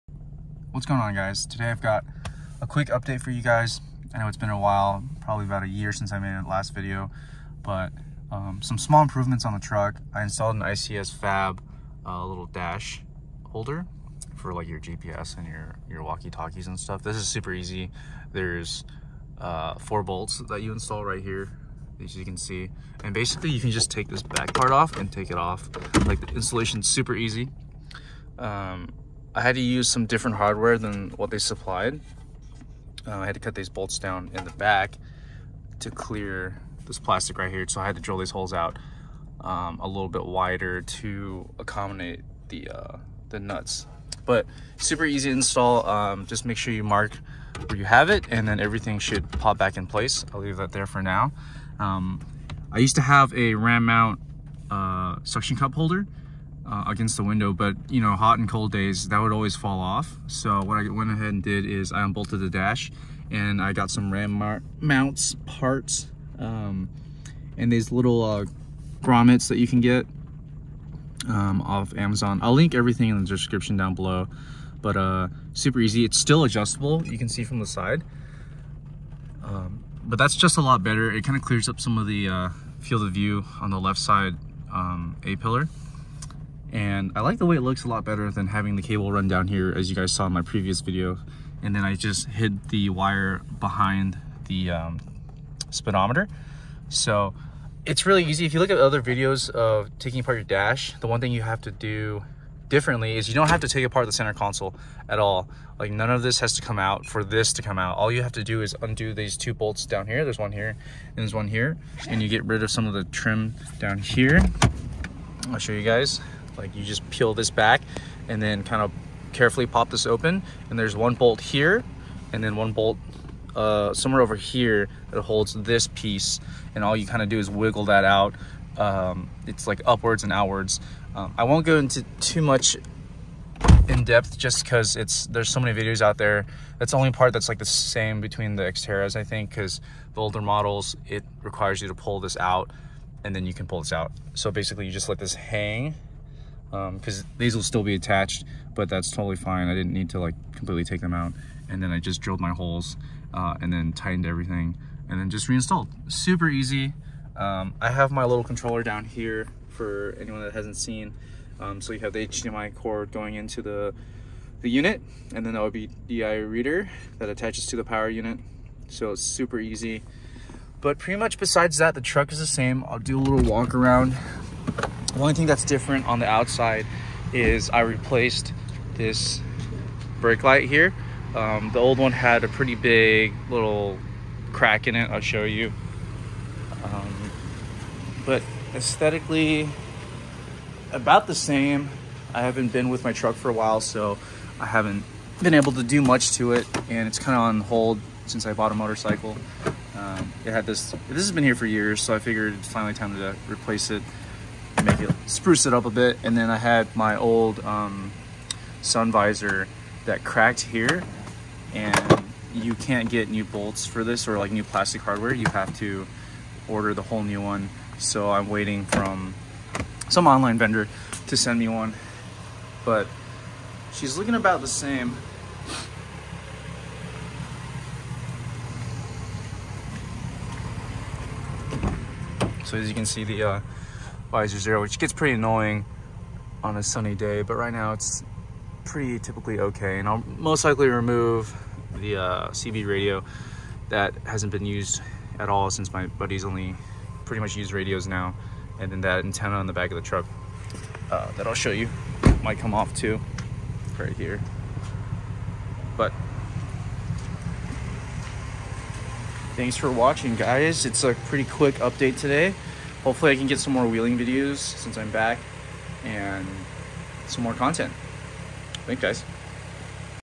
Bullydog Ram Mount, ICS Fab mdash mount l Nissan Frontier
Hey everyone!
Its been awhile but here a small update on what I've added,
I've been away from he truck for awhile so no new content besides these small improvements/fixes/ mods here  are  the links to the products i used in the video.

Parts:
Rubber Gromets

RAM-B-201U-A  UNPKG RAM DBL SOCKET ARM B BALL A LENGTH

 RAM-B-202U-12  RAM BASE 1" X 2" W/ 1" BALL

I re used my bully dog ram mount bracket that hooks into the back of the bully dog.  unfortunately i dont have that part number